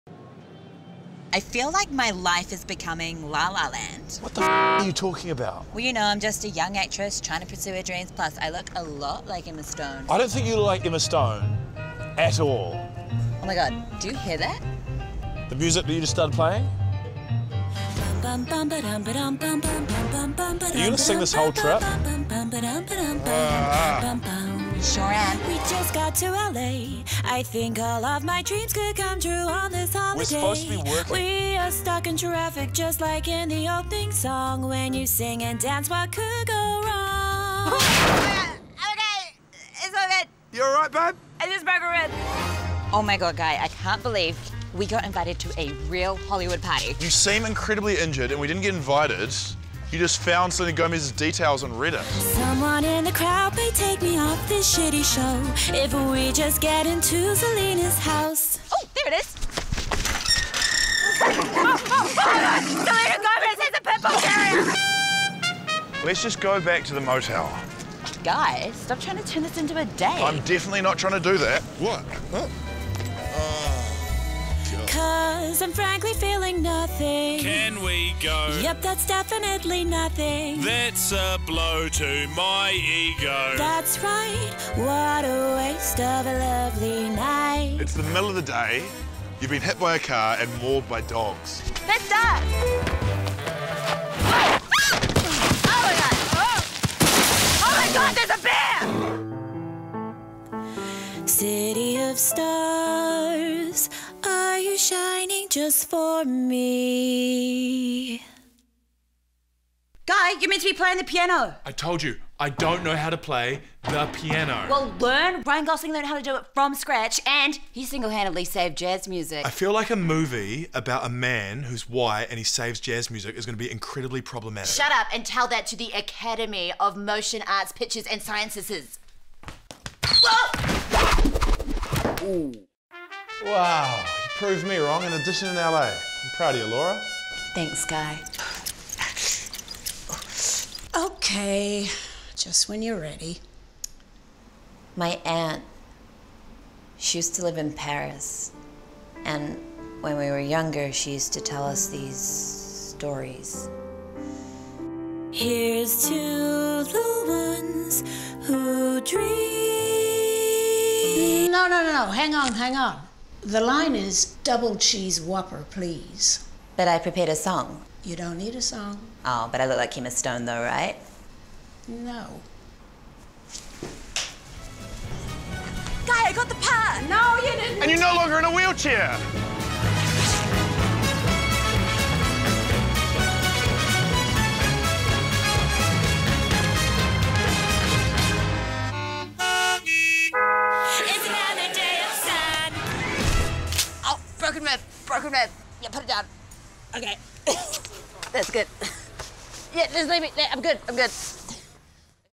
LA LAura in La La Land
Laura Daniel and Guy Williams recreate LA LA Land